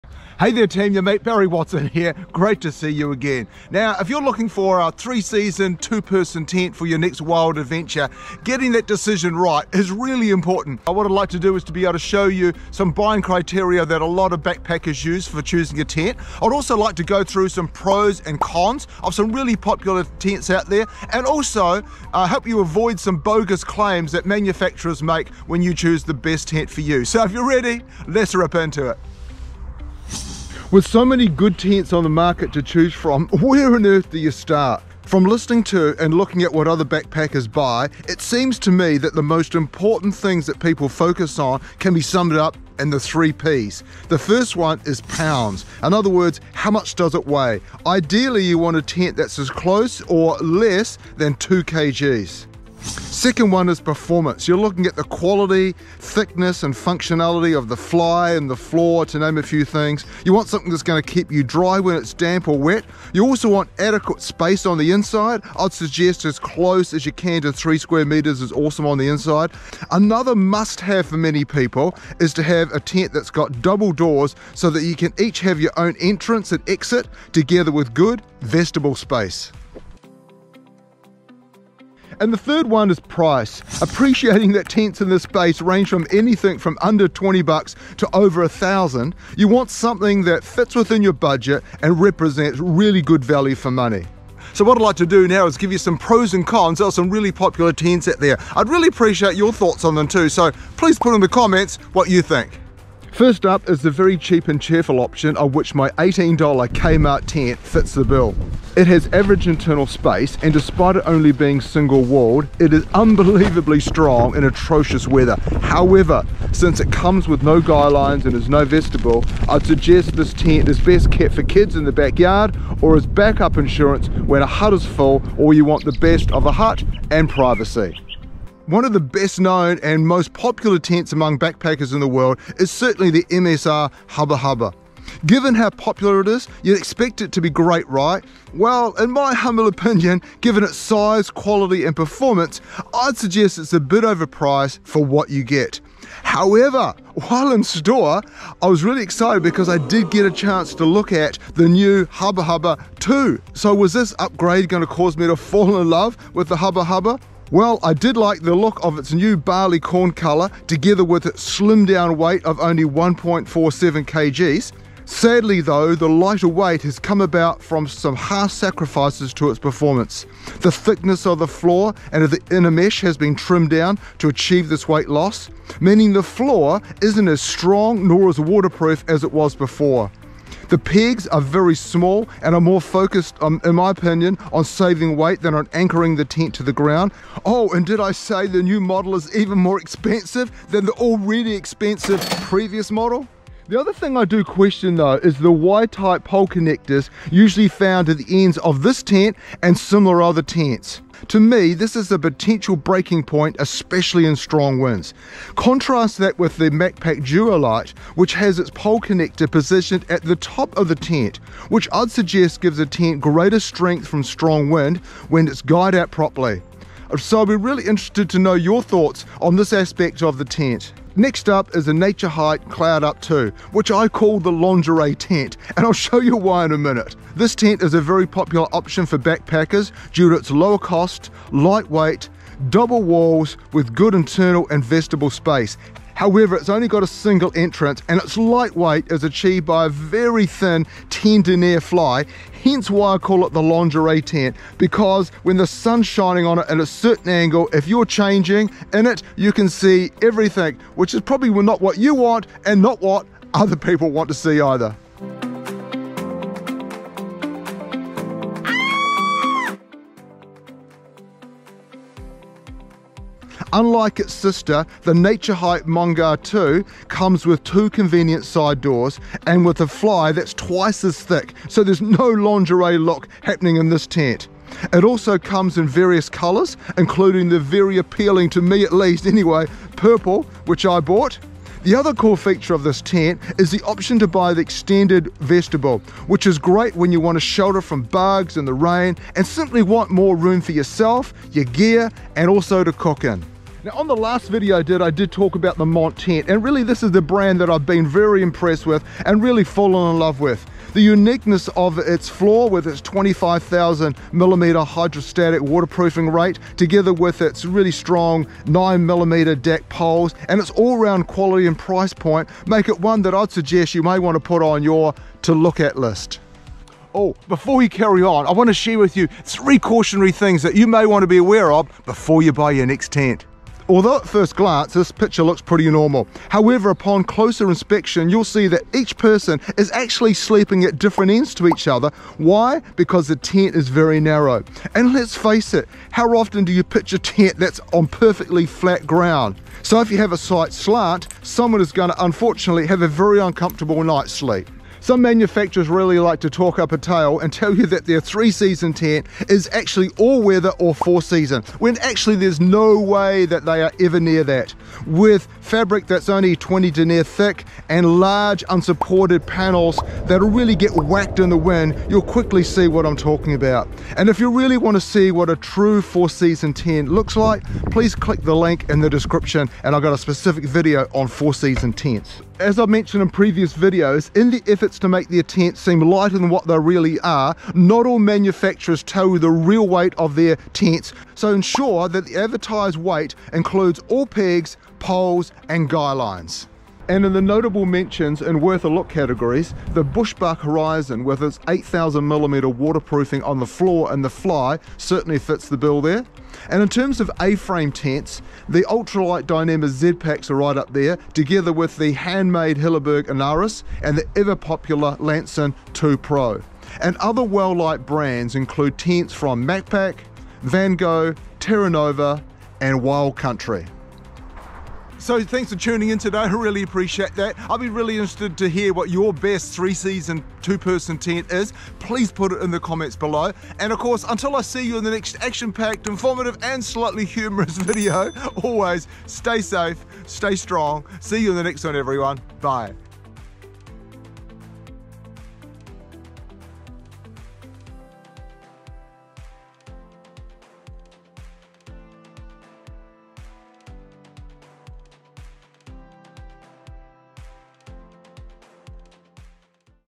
Best 2Person Hiking Tents (Real Examples)
When you’re looking for a 2 person tent, for backpacking, or hiking, you want to get it right. Check out the short reviews on do on several well-known tents, together with 3 bogus claims tent manufacturers make that you want to be aware of.

Thanks to Bivouac Store in Christchurch for letting me film their tents.

✅ NEXT...

I look forward to any questions or comments you may have.

Barry Watson